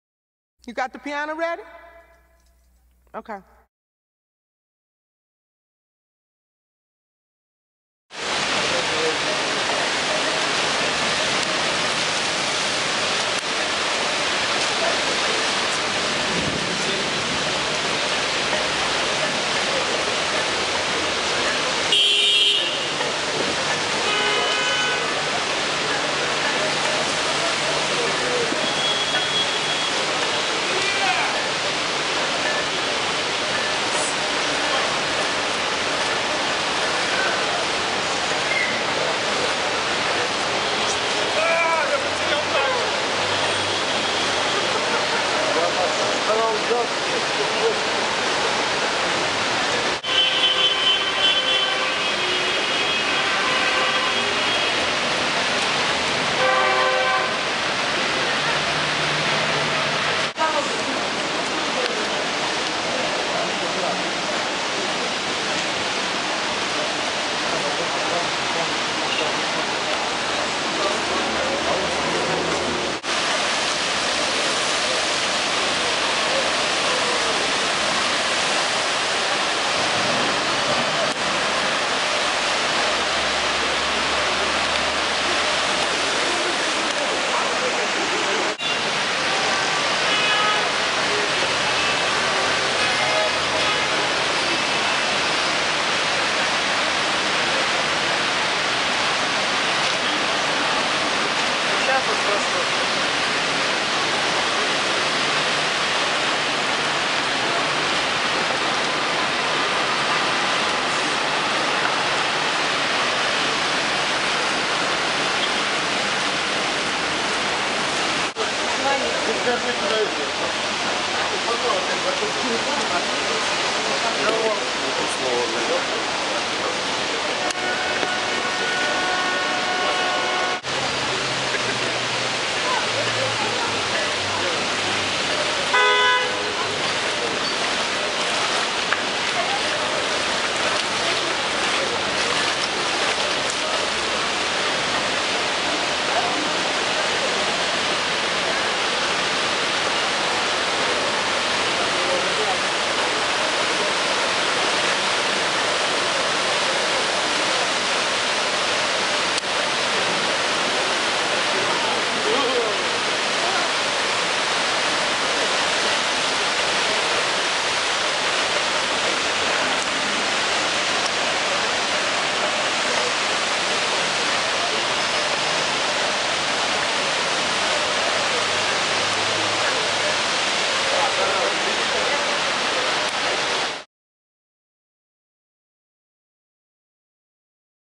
Наводнение в центре Москве 31.07.2011
После прошедшего в 16 часов дождя, рядом с выходом из метро "Белорусская-радиальная" образовалась большая лужа. Подтопленный путепровод создавал трудности для автомобилей и для движения пешеходов.